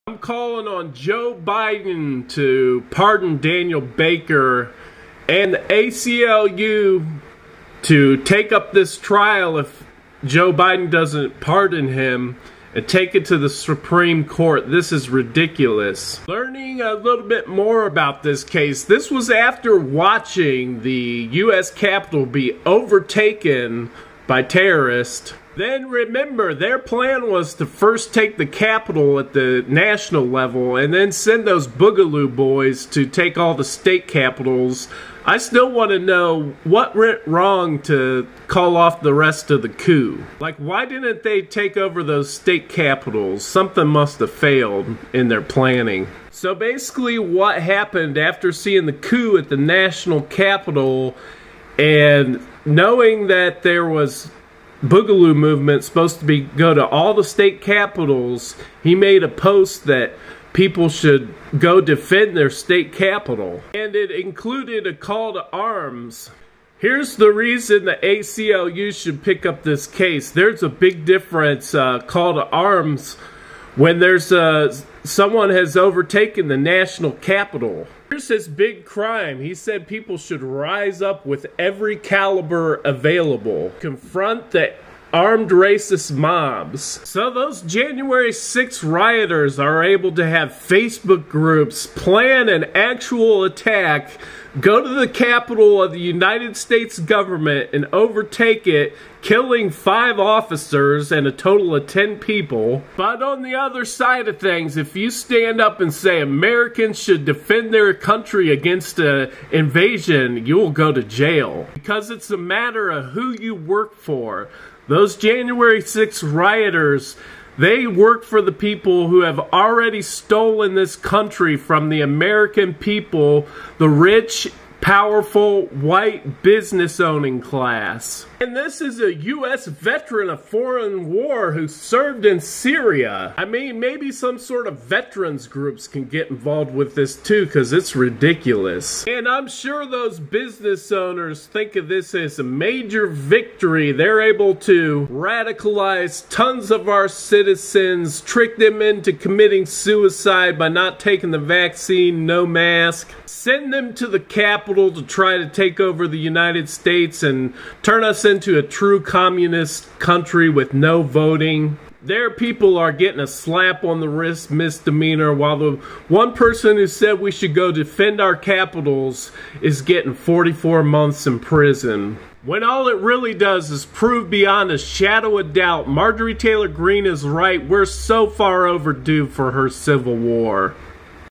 freeDanielBaker calling on Joe Biden ACLU to pardon Daniel Baker Jan 6th Attack Trump Coup Capital
I talk about the disgusting asymmetrical way in which events were processed concerning the Jan 6 Donald Trump Terrorist attempted Coup of the US Government at the national Capital. While right wing radical racist terrorists were given their own facebook groups, allowed to plan and execute an attack on the US Capitol killing cops and almost 10 people total,,, Daniel Baker was arrested for suggesting that Americans should defend their democracy.  The real crime he committed was not working for the rich white business owners who have already completely and totally stolen this country from the everyday people.

Joe Biden should absolutely pardon him.  After seeing the coup on T.V. that changes everything about a call to arms when there's real occupation.  His long sentencing while the actual people who showed up with machineguns at the capitals aren't charged with anything only proves how correct Marjorie Taylor Greene is concerning the "American Divorce" civil war.  I bet it was also never investigated how he was getting harassed by people who are likely part of an actual terrorist group.

freeDanielBaker calling on Joe Biden ACLU to pardon Daniel Baker Jan 6th Attack Trump Coup Capitol